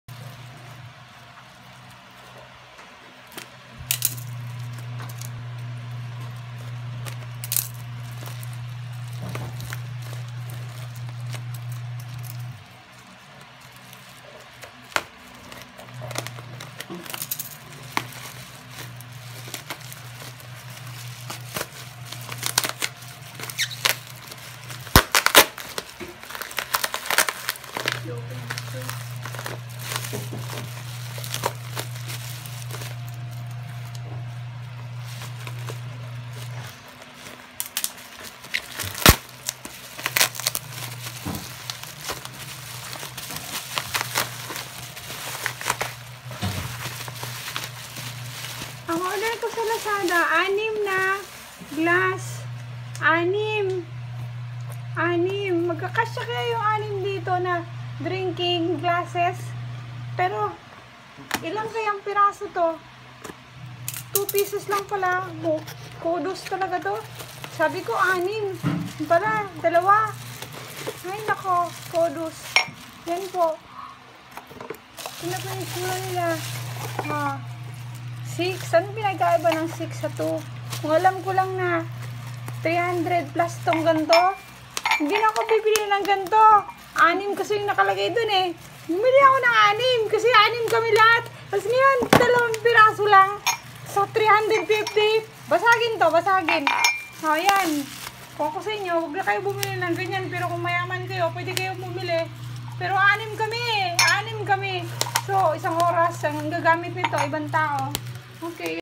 Unboxing dring glass
I ordered 6 pieces but arrived 2 pcs only.